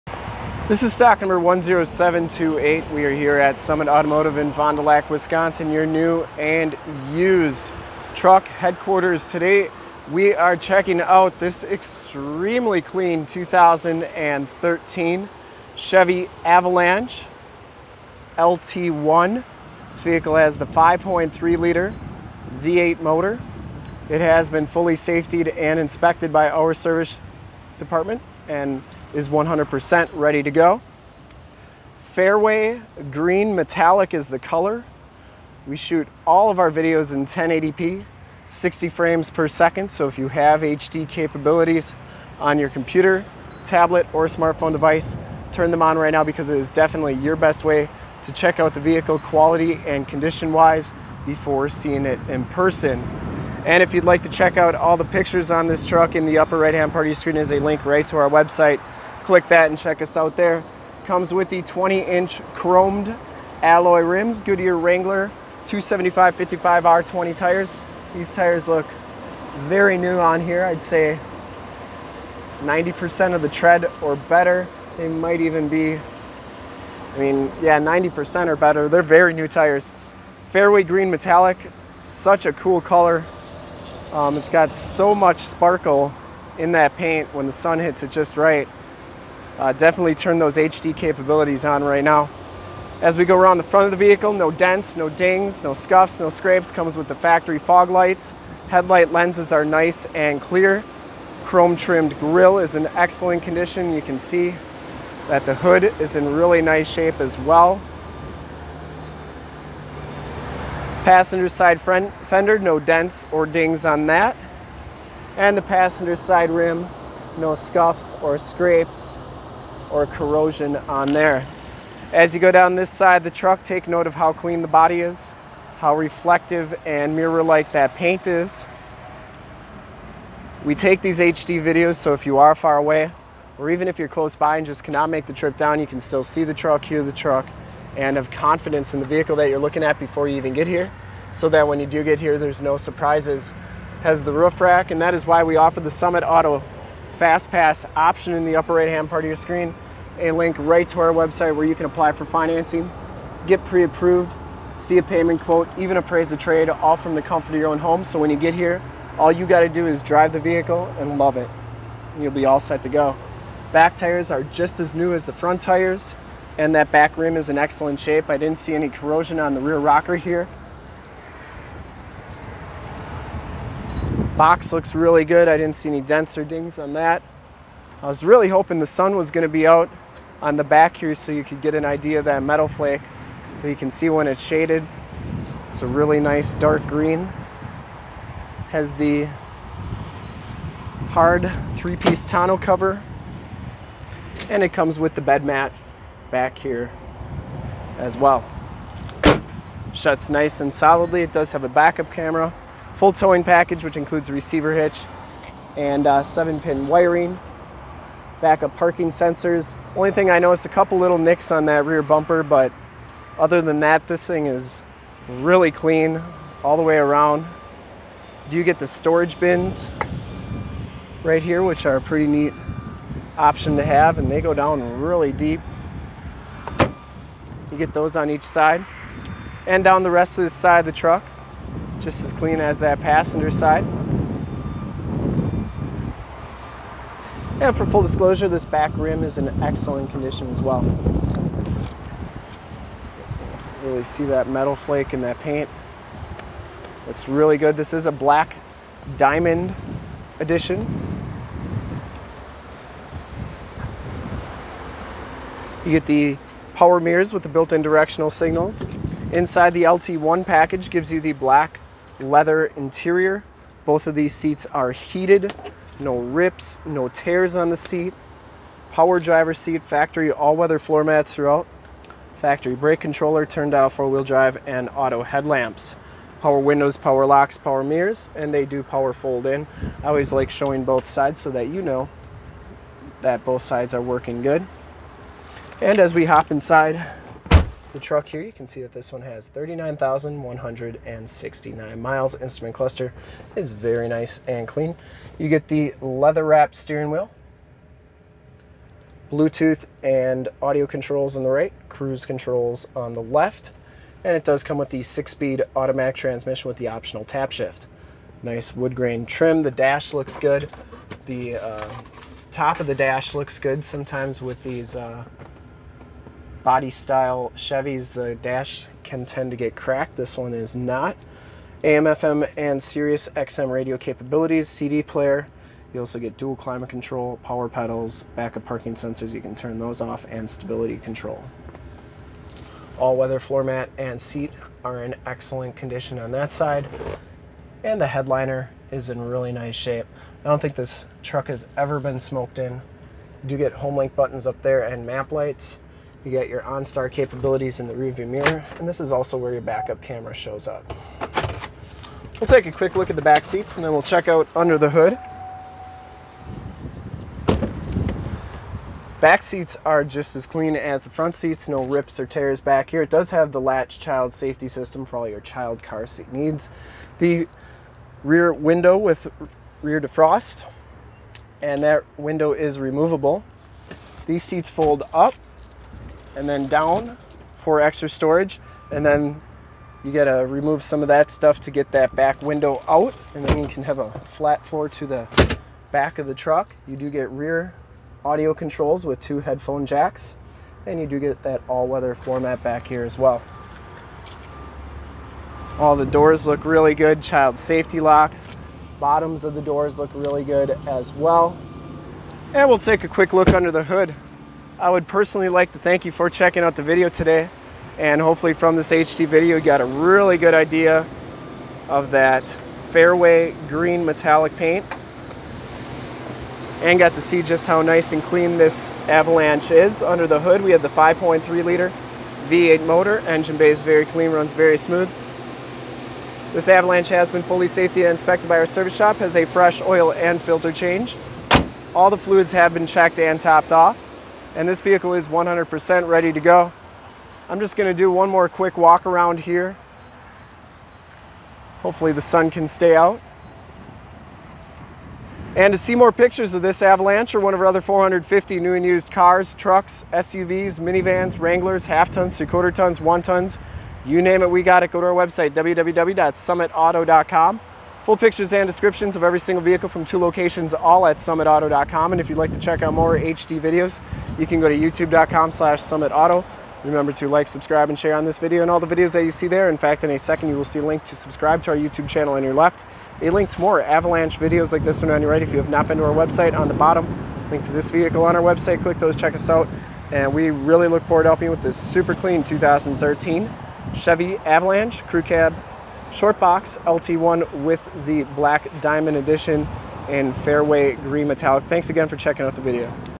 2013 CHEVROLET AVALANCHE RARE FAIRWAY GREEN METALLIC WALK AROUND REVIEW 10728 SOLD!
This 2013 CHEVROLET AVALANCHE LT1 BLACK DIAMOND IN FAIRWAY GREEN METALLIC is the vehicle we did walk around review of today.

Thank you for checking out this video of this 2013 CHEVROLET AVALANCHE LT1 BLACK DIAMOND IN FAIRWAY GREEN METALLIC

STOCK: 10728
PRICE: $25,999
MILES: 39,149
MAKE: CHEVROLET
MODEL: AVALANCHE
VIN: 3GNTKFE75DG248284
LOCATION: FOND DU LAC OSHKOSH WISCONSIN, 54937 TRUCKS ON 41

1 OWNER! CLEAN TITLE HISTORY! 5.3 Liter V8 Engine with Active Fuel Management, 320 Horsepower, E85 Capabilities Flex Fuel Vehicle, Full Four Door Crew Cab, Short Box 5.3 Foot Bed, LT1 Package 1LT LT, Black Diamond Package, 6 Speed Automatic Transmission with Optional Manual Tap Shift, Turn Dial 4x4 Four Wheel Drive 4WD, Reverse Backup Camera in Rearview Mirror, Onstar System, Dual Power Heated Seats, Non Smoker, Black Ebony Leather Seats, Bucket Seats, Factory Hard Plank Tonneau Cover, Full Towing Package with Receiver Trailer Hitch, Wiring and Transmission Cooler Tow Package, Factory Brake Controller, Power Fold in Power Mirrors with Built-in Directional Signals, Stabilitrak Traction Control, 3.42 Gears with Automatic Locking Differential Limited Slip Differential, Goodyear Wrangler Sr-A 275/55 R20 Tires, 20 Inch Rims Premium Wheels, Chromed Alloy Rims Premium Wheels, Four Wheel Disc Brakes, Factory Running Boards, Bedmat, Reverse Sensors, Roof Rack Luggage Rack, Locking Tailgate, Chrome Trimmed Grill, Chrome Trimmed Mirrors, AM / FM Radio Tuner, Sirius/XM Satellite Radio Capabilities Sirius / XM, CD Player, Bose Premium Audio Sound System, Bluetooth, Hands-Free Phone Controls Blue Tooth, Auxiliary MP3 Jack Portable Audio Connection, Keyless Entry with Factory Remote Start, Rear Window Defroster, Removeable Rear Window, Adjustable Height Seatbelts, Driver and Passenger Front Air Bags, L.A.T.C.H. Child Safety System, Side Curtain Air Bags SRS Safety Restraint System, Multi-Function Steering Wheel Controls, Homelink System with Three Programmable Buttons for Garage Doors, Lighting Systems & Security Systems, Compass, Outside Temperature Display and Mileage Display, Power Adjustable Pedals, Factory All Weather Floormats, Woodgrain Dash And Door Trim, Air Conditioning AC, Cruise Control, Power Locks, Power Windows, Tilt Steering Wheel, Automatic Headlights Autolamp, Fairway Green Metallic, ONE OWNER! CLEAN AUTOCHECK! Very very clean inside and out! This is one of the sharpest 2013 Chevrolet Avalanche Crewcab shortbox 1/2 ton trucks we have ever had on our lot! Make your move before this super clean 4wd is gone!

STOCK: 10728
PRICE: $25,999
MILES: 39,149
MAKE: CHEVROLET
MODEL: AVALANCHE
VIN: 3GNTKFE75DG248284